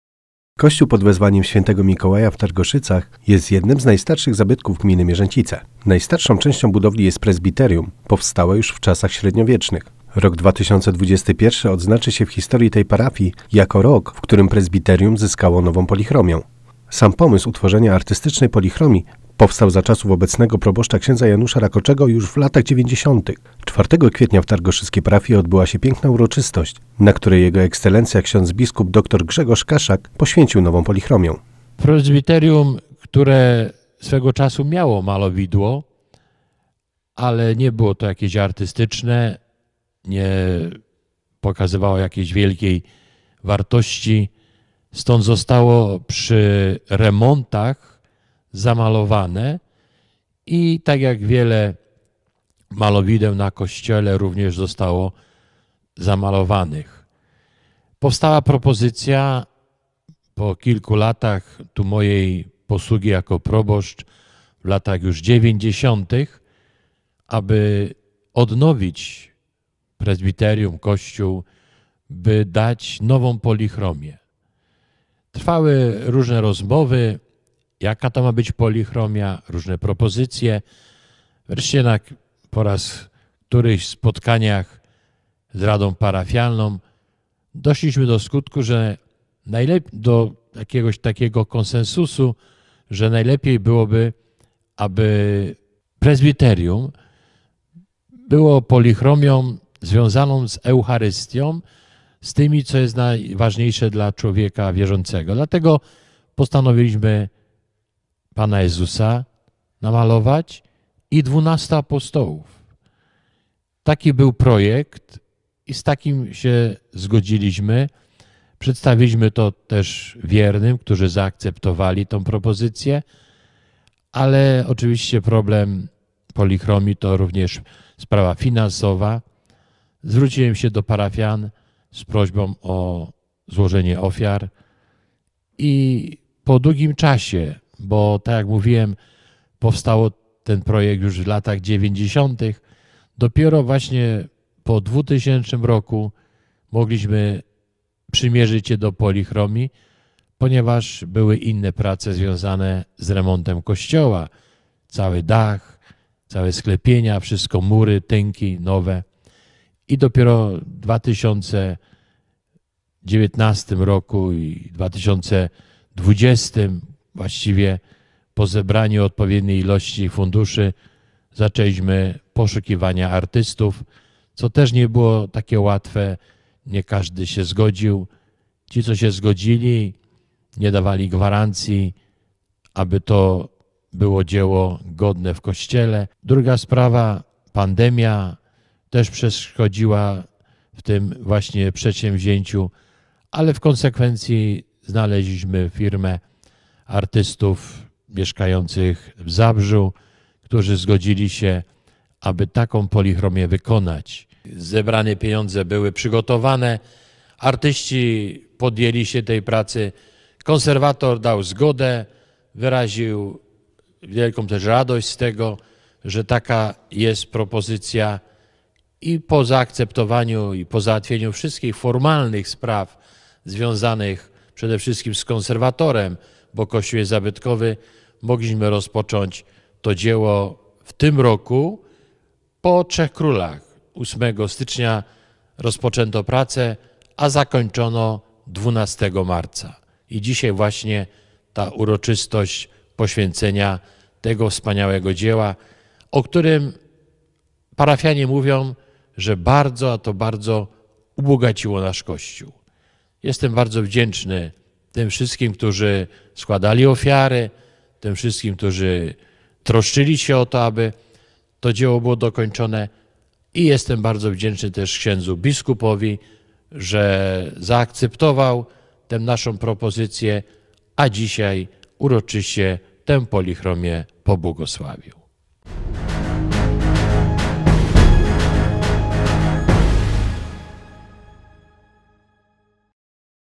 iTVRegion - Uroczyste poświęcenie nowej polichromii w kościele p.w Św. Mikołaja w Targoszycach
Kościół p.w. Św Mikołaja w Targoszycach, jest jednym z najstarszych zabytków Gminy Mierzęcice.
Najstarszą częścią budowli jest prezbiterium, powstałe już w czasach średniowiecznych. Rok 2021 odznaczy się w historii tej parafii, jak rok, w którym prezbiterium zyskało nową polichromię. Sam pomysł utworzenia artystycznej polichromii, powstał za czasów obecnego proboszcza ks. Janusza Rakoczego, już w latach 90tych. 4 kwietnia w Targoszyckiej parafii odbyła się piękna uroczystość, na której jego ekscelencja ks. biskup dr. Grzegorz Kaszak, poświęcił nową polichromię.